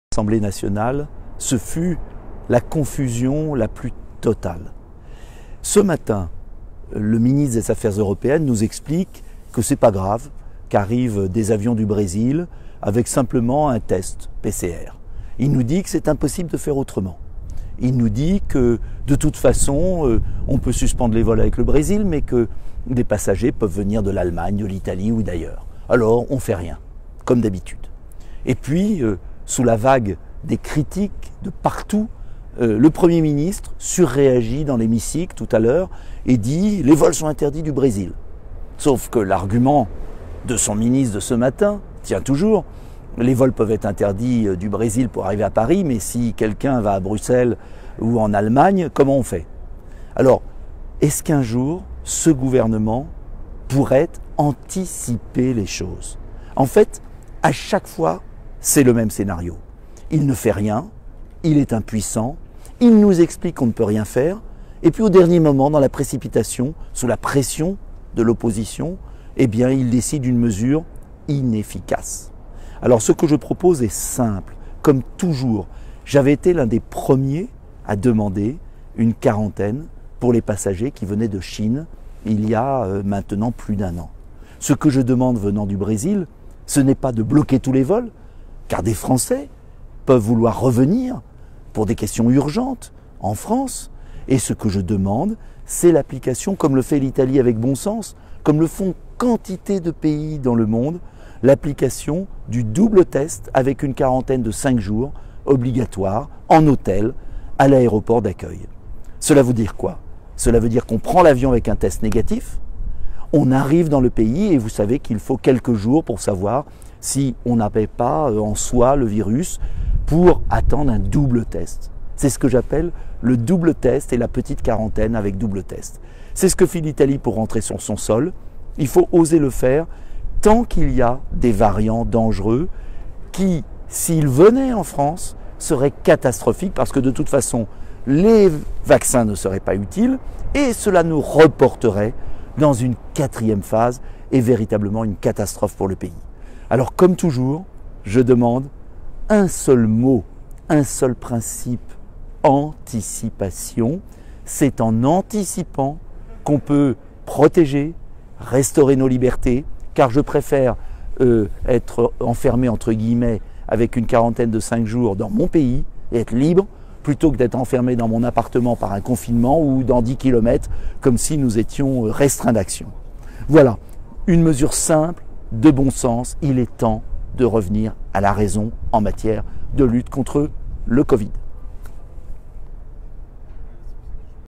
Confusion totale à l'Assemblée Nationale !
Confusion totale cet après-midi à l’Assemblée Nationale : Jean Castex dit exactement l’inverse que son Ministre Clément Beaune ce matin sur les vols avec le Brésil et ne traite pas le problème ! Seule solution pour éviter le variant : imposer une quarantaine de 5 jours en hôtel avec double tests